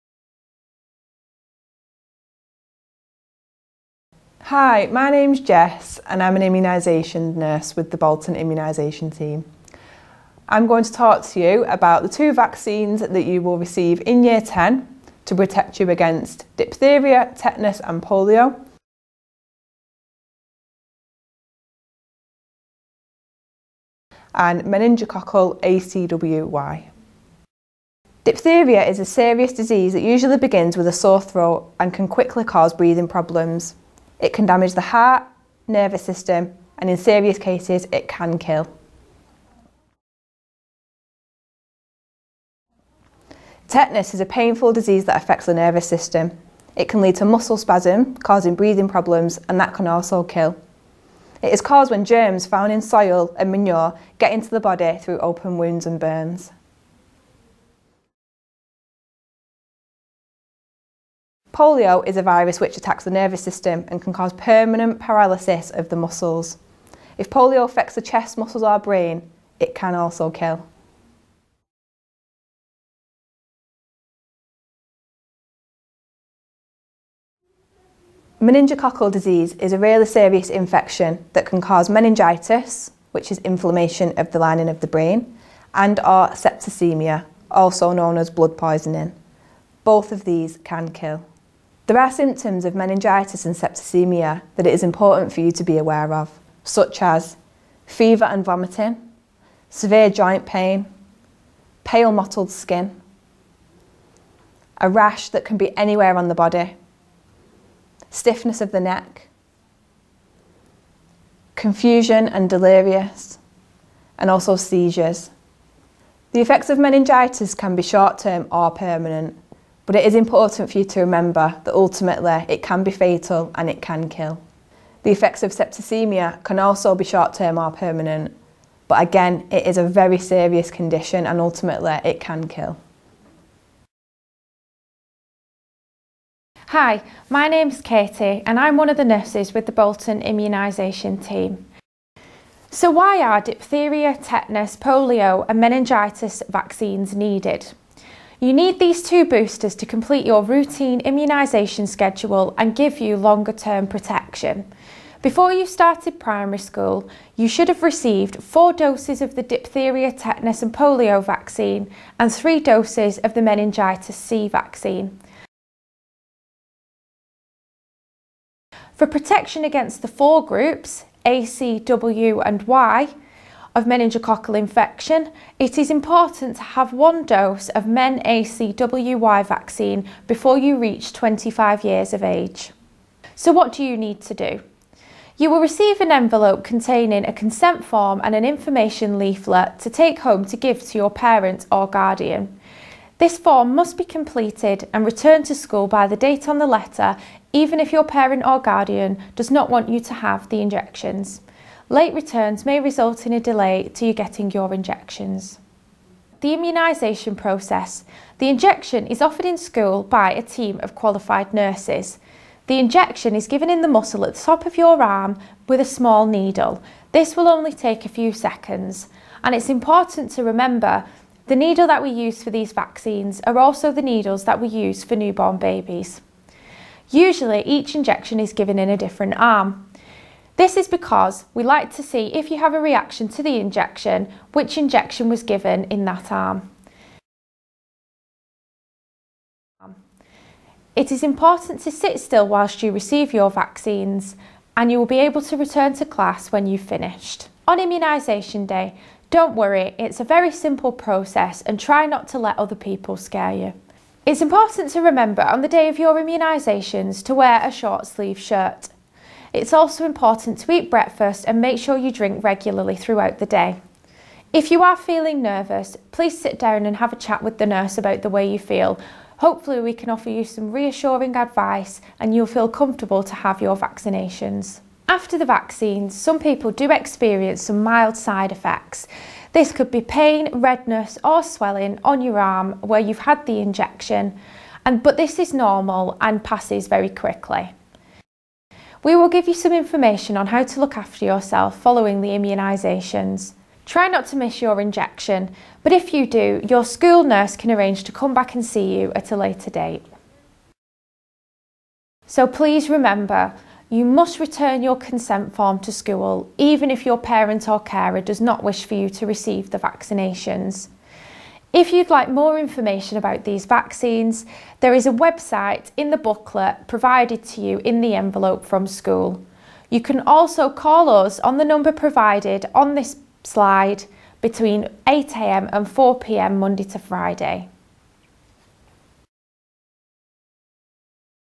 Tetanus, Diphtheria, Polio and Meningitis ACWY vaccinations in school (with BSL Translation)
Bolton NHS Foundation Trust explains receiving your Tetanus, Diphtheria, Polio and Meningitis ACWY vaccinations in school with BSL Translation.